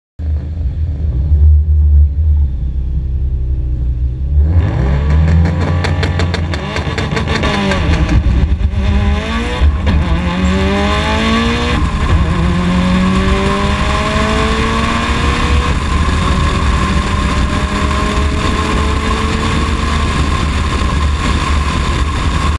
12.3 @ 114mph (Street Tires 24lbs) Rear Cam
Ive been trying to get my drivermod back to where it was before I broke my transmission.

I haven't driven the same since.

I've been practicing smooth disengagement and shifting the last few months.

Whole night was pretty good. Went A to B, missed no gears, etc.

BUT, this run, when I shift to 2nd I wasn't synced up. You can hear it. It dragged into gear. I gotta work more on my shifting.

But, for my street tires and bad weather, I ran pretty good. Only 1 tenth off my best ET with street tires. Hoping to go out again with better weather and maybe crack 11s on street tires.

I think I figured out better shift points as well :) can't wait to try em out!